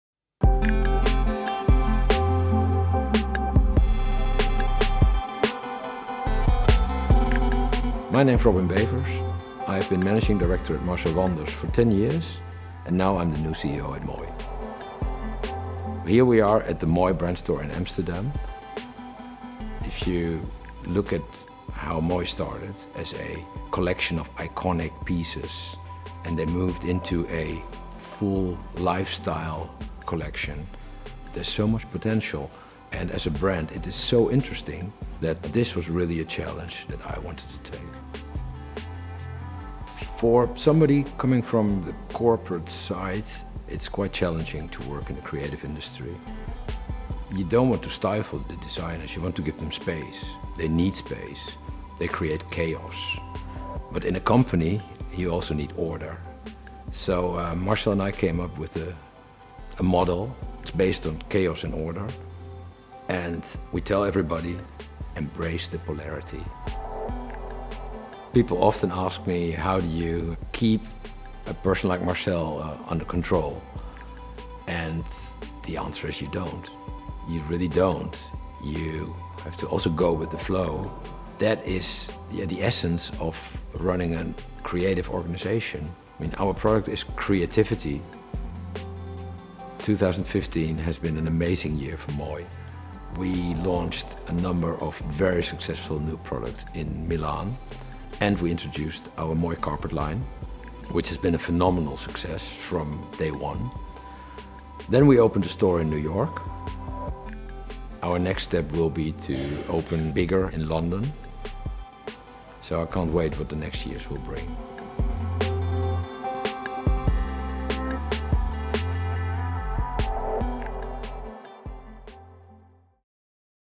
Running a design brand is about "embracing chaos," says new Moooi CEO
Running a successful design company is about striking a balance between chaos and order, Robin Bevers claims in his first video interview since taking over as CEO of Dutch brand Moooi.